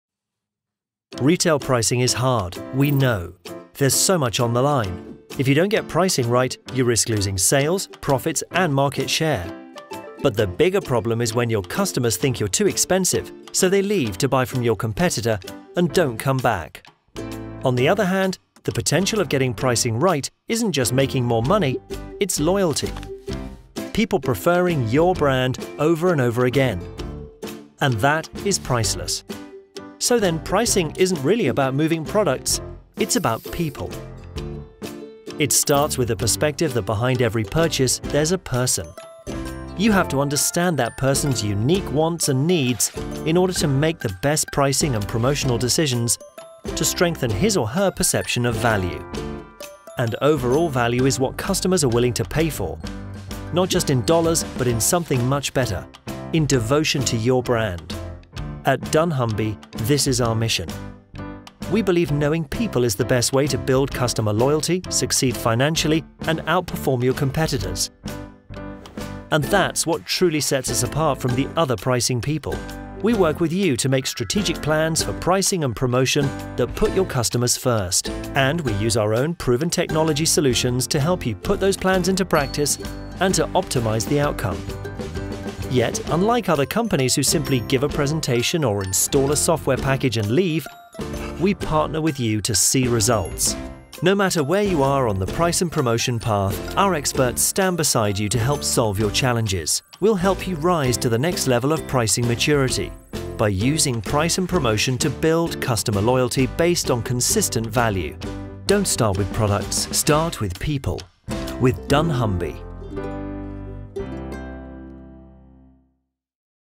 Customer-first pricing and promotion from dunnhumby
We’re not like the other pricing people. Our plans begin with the perspective that, behind every purchase, there's a person. You have to understand that person's unique wants and needs in order to make the best pricing and promotional decisions to strengthen his or her perception of value and earn their lasting loyalty. Our expert consultants know what it takes to get your price and promotions performance to the next level, and our powerful software can help you make quick, strategic decisions along the way.